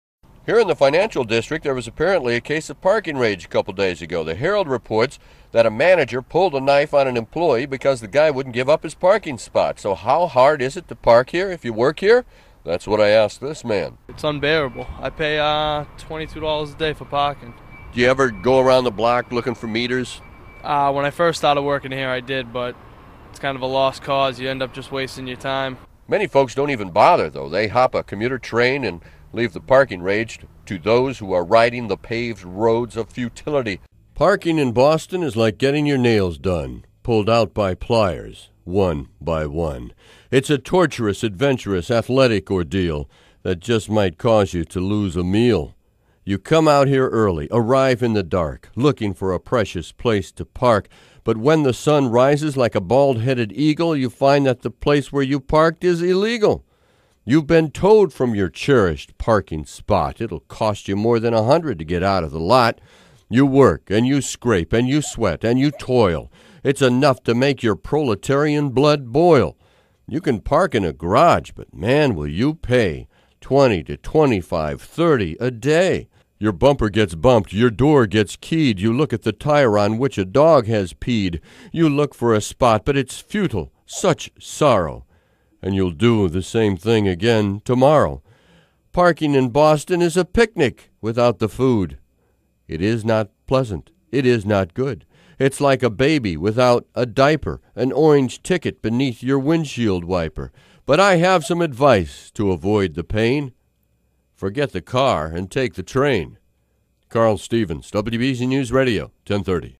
Parking in Boston: A Story and a Poem
Here's a story and a poem, highlighting the challenge of parking in Boston.